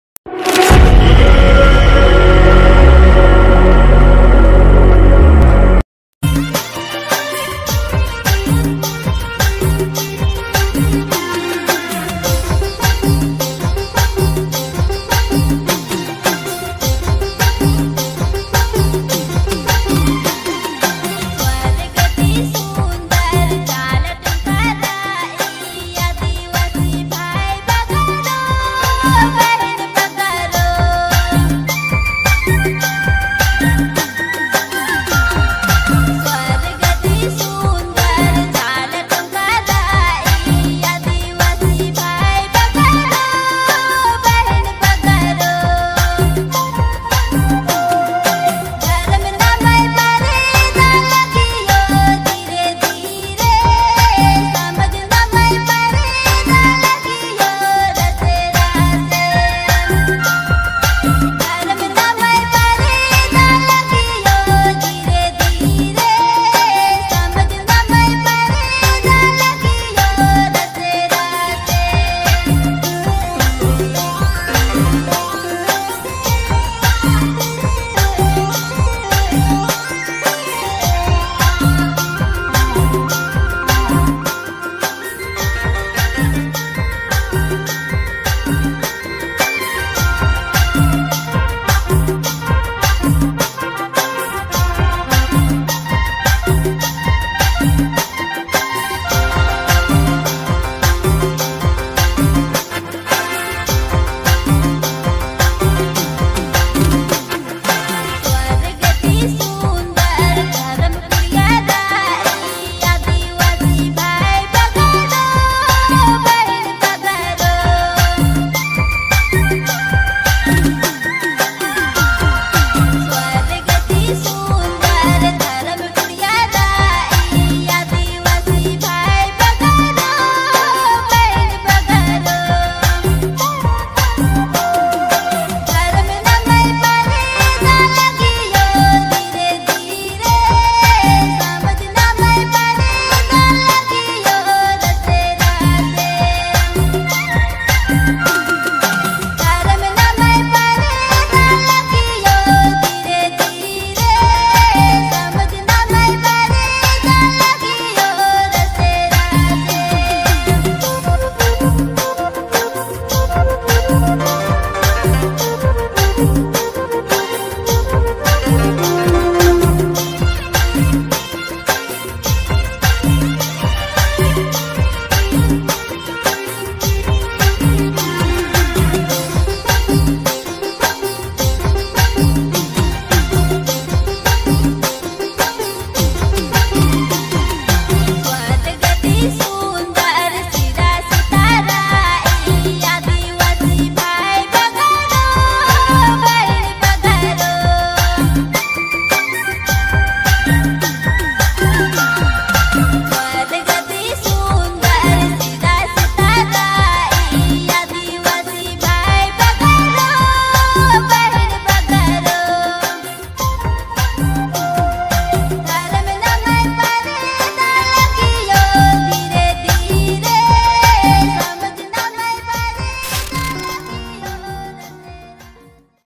Adivasi Group Dance||VLOG VIDEO
Sarna bhajan song Mein Aadivasi dance 2020 liya

Ranchi piska mode Jatra Maidan

YOU VLOG